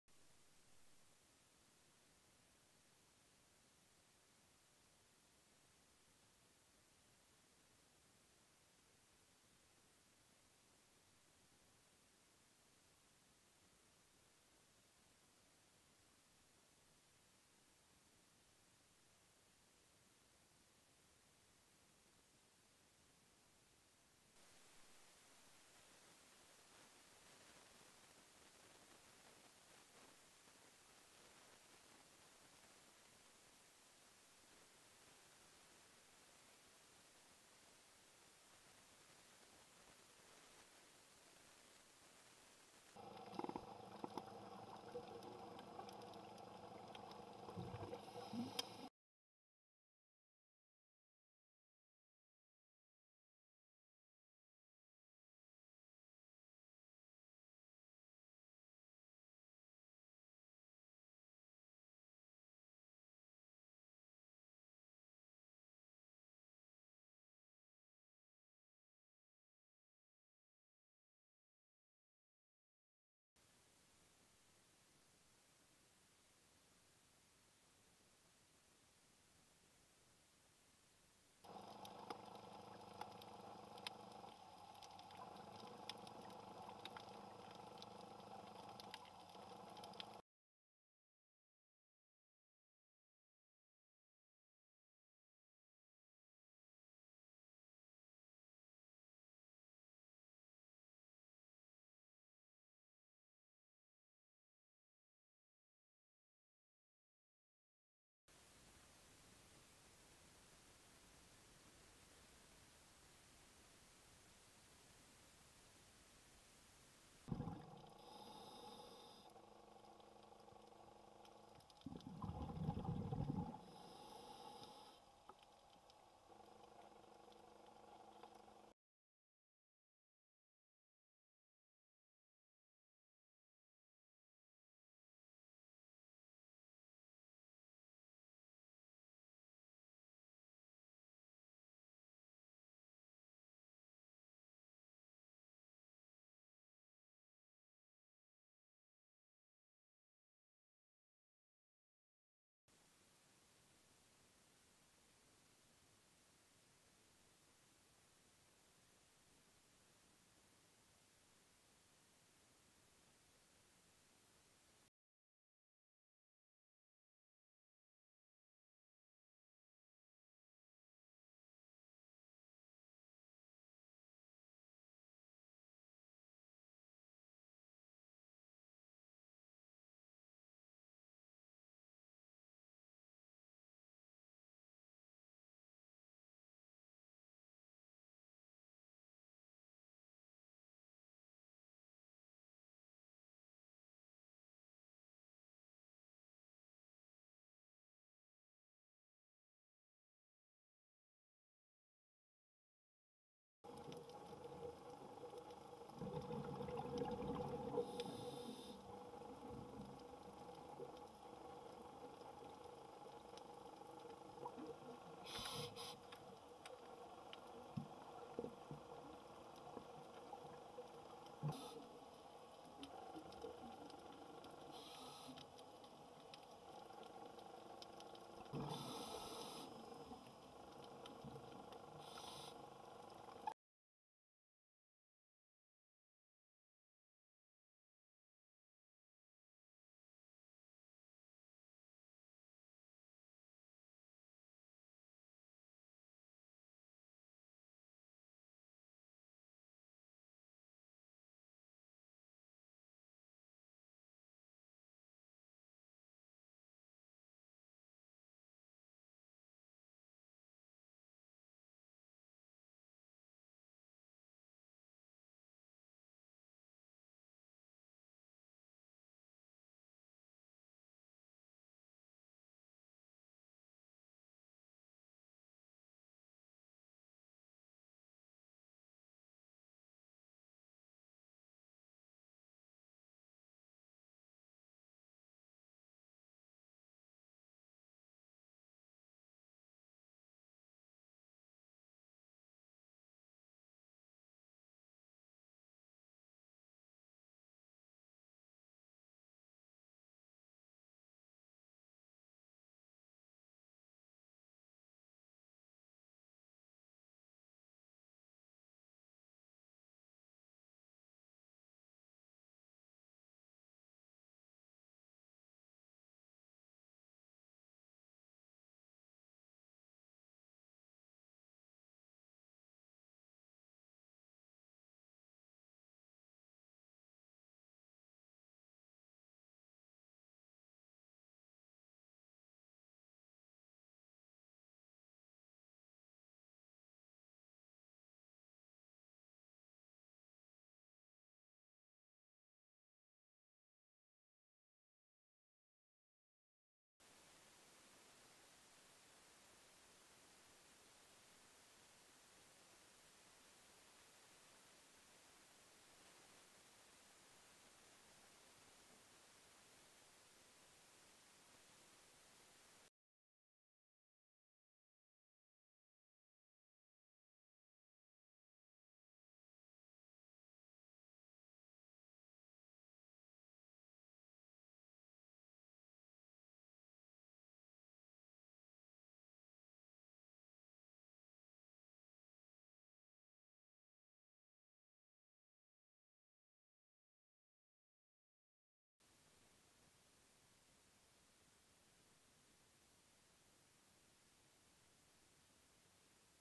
Need a Break? Spend a little time underwater!
Beautiful underwater scenery to remind us all why we love diving so much.